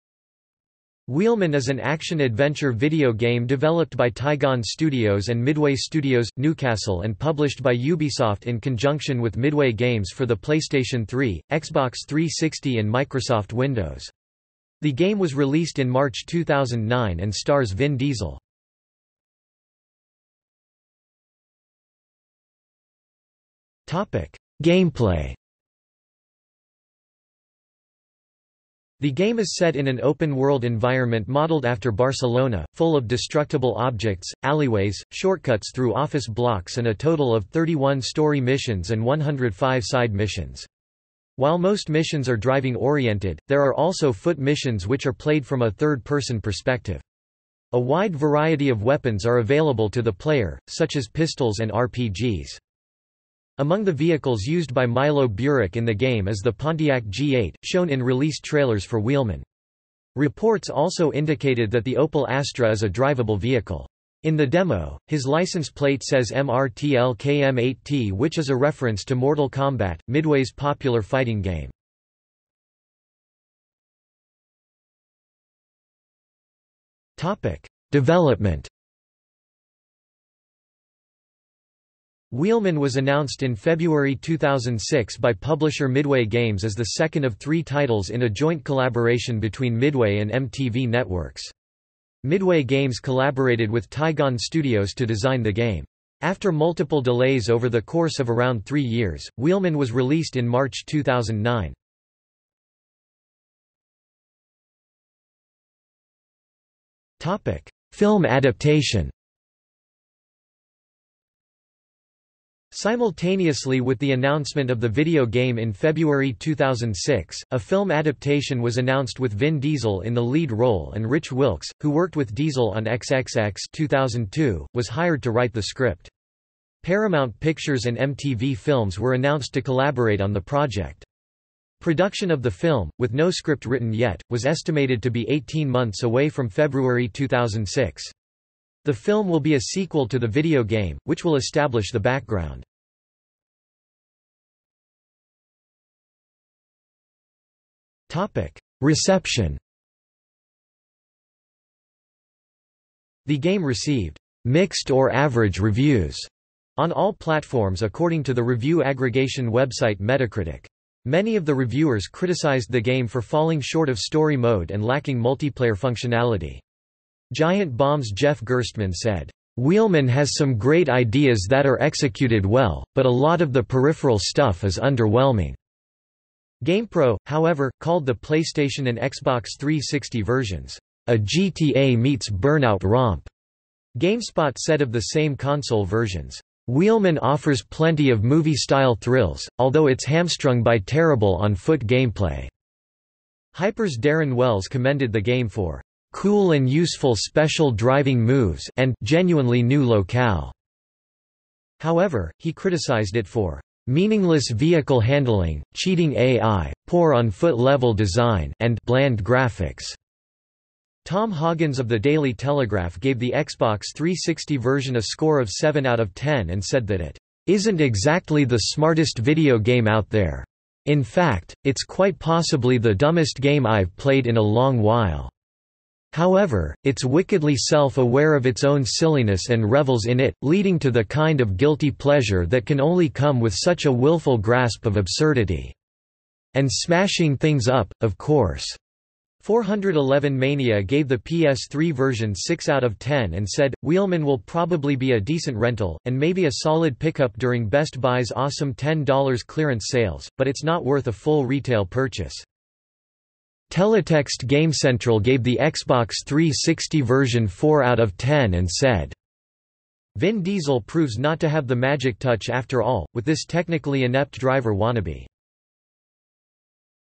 Wheelman (video game) | Wikipedia audio article
00:00:20 1 Gameplay
00:01:13 2 Development
00:01:43 3 Film adaptation
00:02:24 4 Reception

- Socrates

SUMMARY
Wheelman is an action-adventure video game developed by Tigon Studios and Midway Studios - Newcastle and published by Ubisoft in conjunction with Midway Games for the PlayStation 3, Xbox 360 and Microsoft Windows. The game was released in March 2009 and stars Vin Diesel.